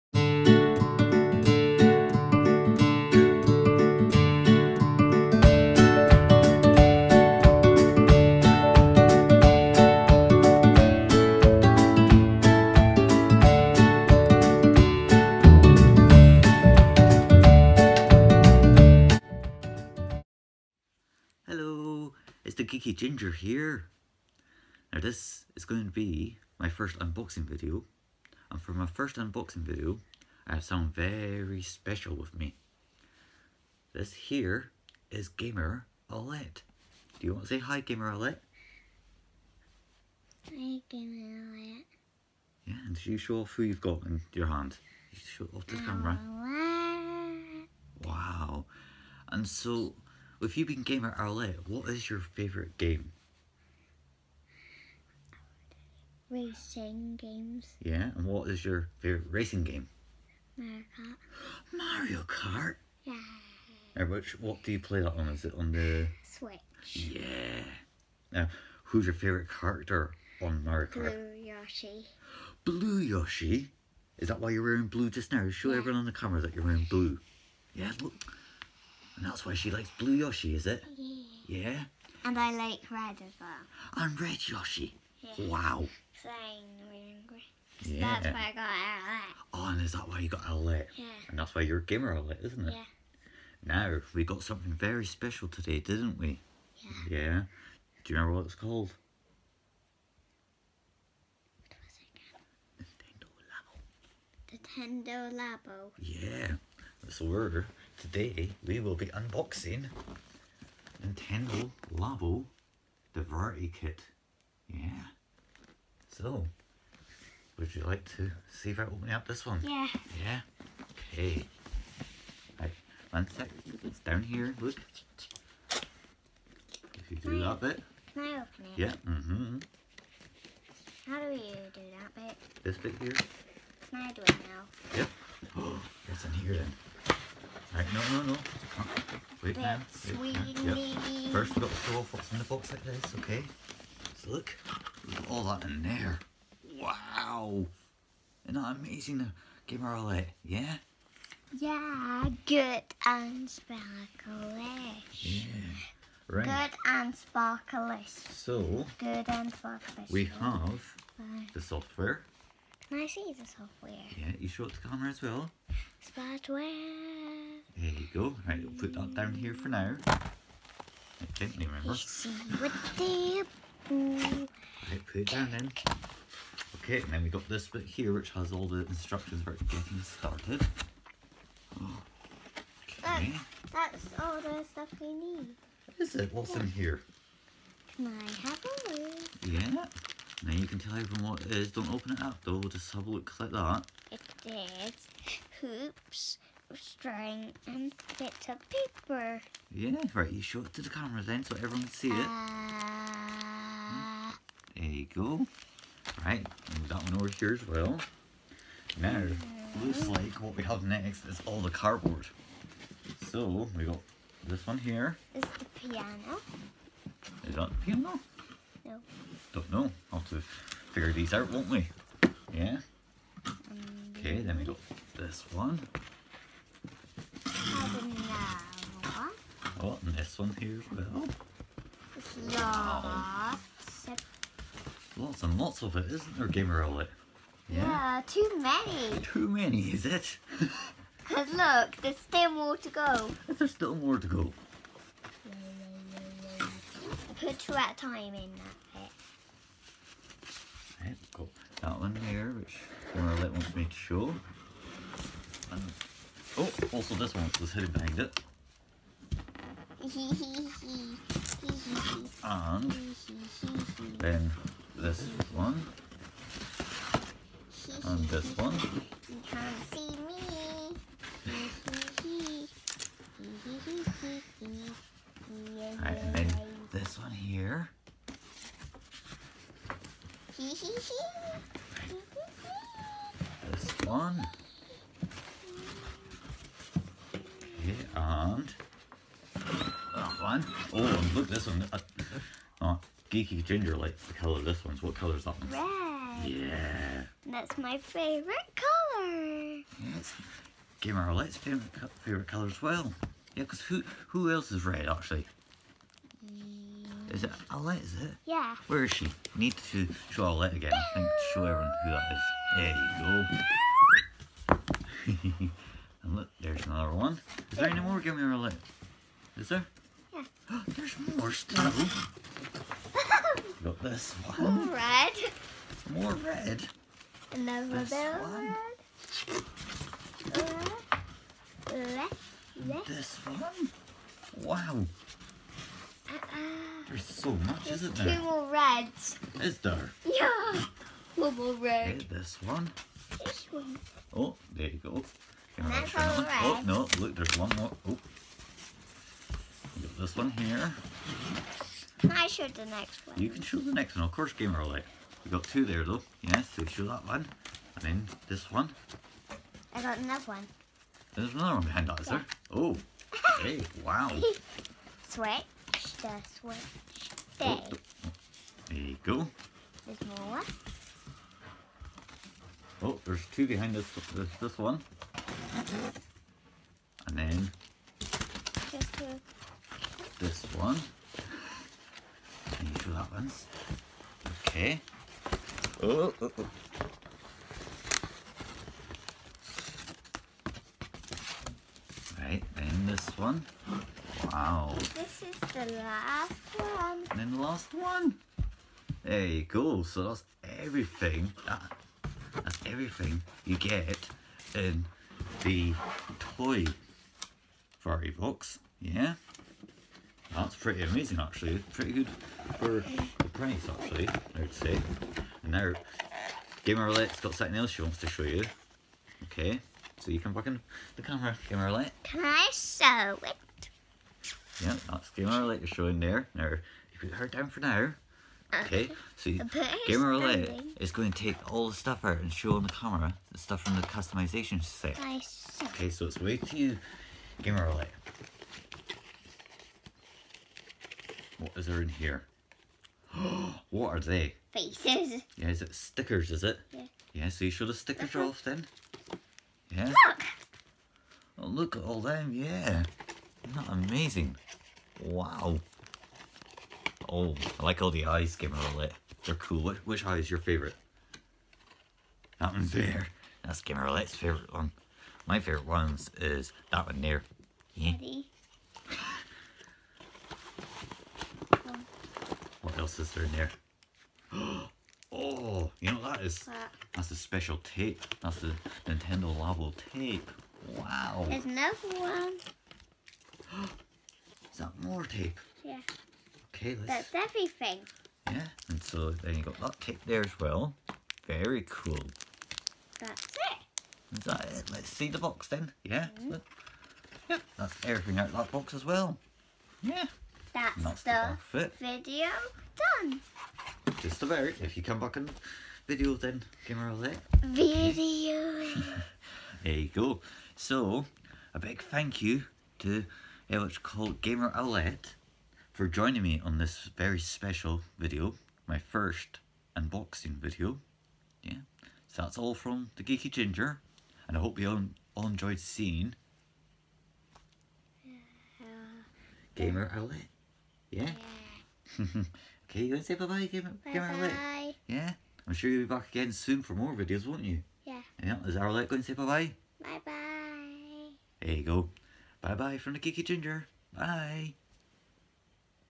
Unboxing Nintendo Labo with someone special !
This is my 1st Unboxing video ! For my 1st ever Unboxing Video I thought it would be great to include someone special. Till next time from The Geeky Ginger !